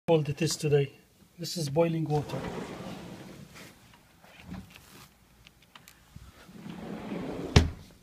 Coldest day of the year 2015
Boiling water turning to snow immediately